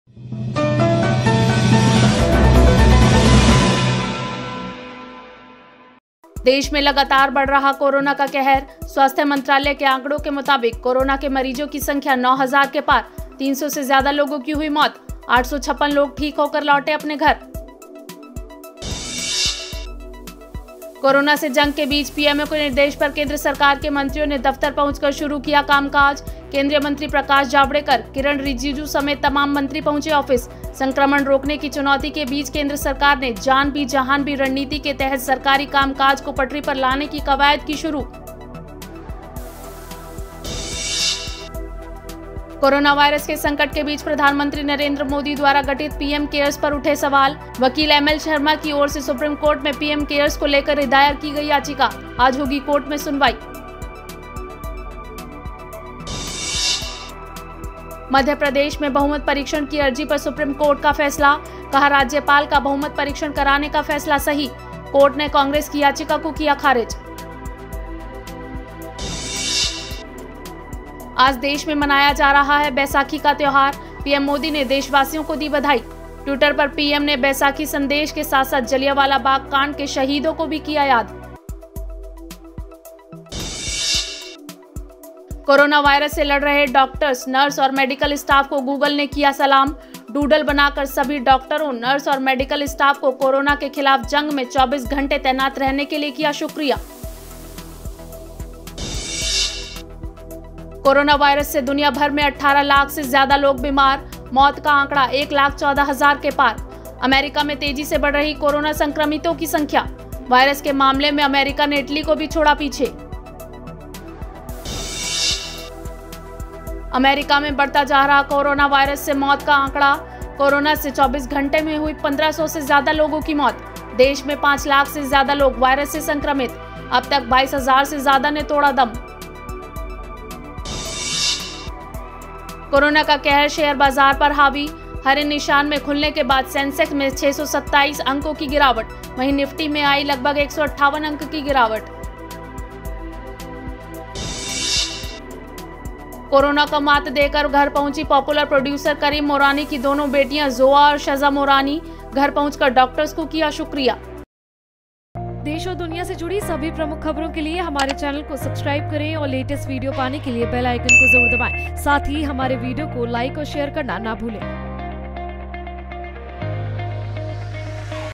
Corona से जंग के बीच PMO के निर्देश पर केंद्रीय मंत्री पहुंचे ऑफिस | HS News | Coronavirus
Description- कोरोनावायरस (Coronavirus) को फैलने से रोकने के लिए देशभर में लागू लॉकडाउन (Lockdown) के खत्म होने से एक दिन पहले मोदी सरकार के कई मंत्रियों ने आज से दफ्तरों से काम करना शुरू किया है. आज से दफ्तर ज्वाइन करने वाले खेल एवं युवा मामलों के मंत्री किरण रिजिजू भारतीय खेल प्राधिकरण पहुंचे. उन्होंने कहा, "केवल वरिष्ठ अधिकारी और जरूरी स्टाफ ही आज से कार्यालय आएंगे. हम कोरोनावायरस से जुड़ी सभी दिशा-निर्देशों का पालन करेंगे."